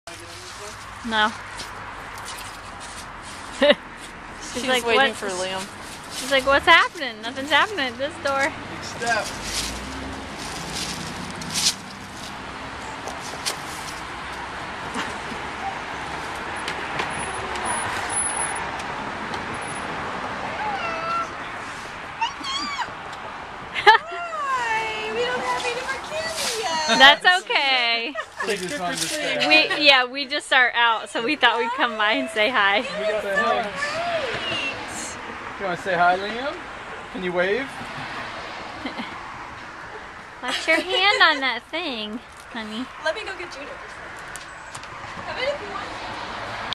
Liam and Joely are getting the hang of trick or treating!
The key, just barge right in, even if they don't have candy!